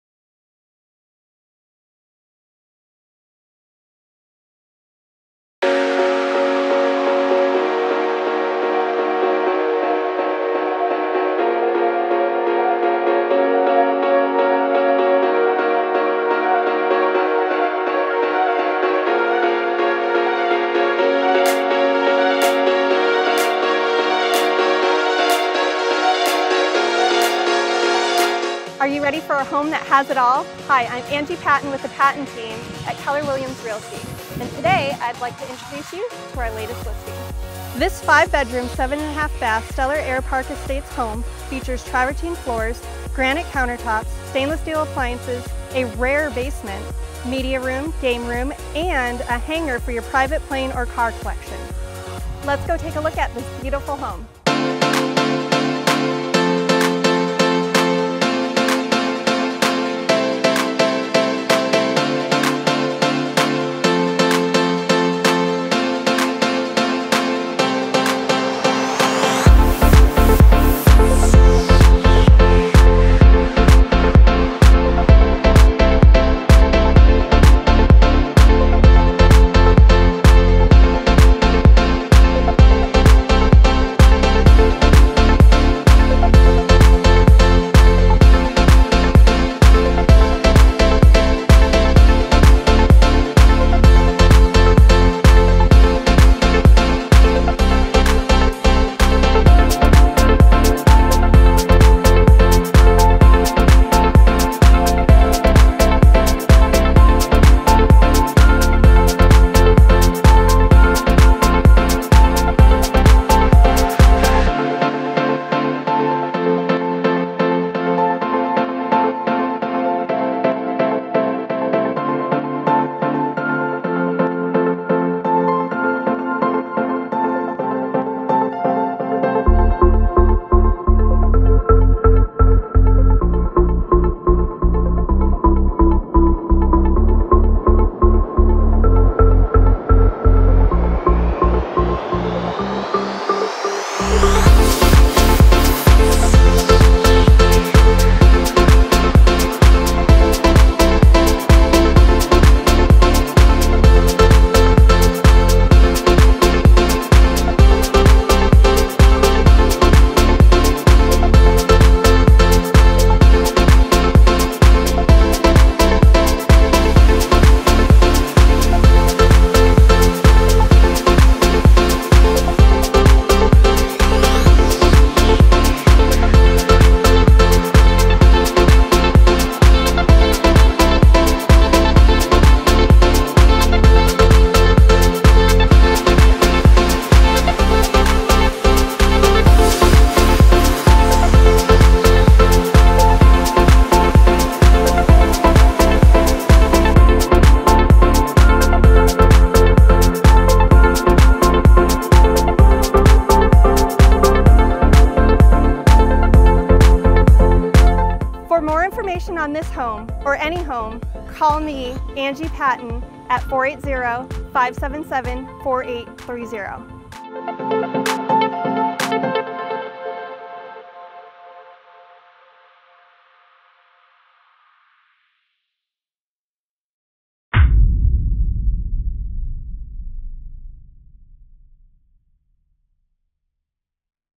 4300 W Earhart Way Chandler Stellar Airpark
PRICED UNDER RECENT APPRAISAL! FURNISHED! BE AIRBORN WITHIN MINUTES! Direct access to property via private taxiway*Stunning custom home perfect for a pilot or car collector! Ideal for entertaining behind the gates of the prestigious Stellar Airpark Estates*Air conditioned & insulated 3 Car garage + Hanger offering RV door, bath w/exit out to pool*Oversized yard with lush grounds, B-I grill, heated pebbletec pool, negative edge spa, grotto, & tranquil waterfall feature*Gourmet kitchen with top of the line appliances, prep sink, granite, custom cabs, double island*Luxurious Master suite offers a sitting room w/fireplace, patio, his/her toilets, vanity, bidet, steam shower, jet tub, massive W-I closet*Basement includes wet bar, media/workout room as well as game/billiards room*

4bedrooms offer full baths & W-I closet*3 Fireplaces*Family room open to kitchen w/Wet Bar*Elegant formal rooms*Coffered ceilings T/O*Grand foyer*5th bedroom/study split from the rest of the home for privacy includes wet bar, dishwasher, wine frig, private balcony*Cul-De-Sac Lot*Within minutes from Chandler Mall, downtown nightlife, restaurant & boutiques, Sky Harbor Airport, Loop 101 and 202, Desert Breeze Park, & schools*Award winning Kyrene school district!

CONTACT THE PATTEN TEAM FOR A PRIVATE SHOWING!
Chris & Angie Patten
Keller Williams Realty East Valley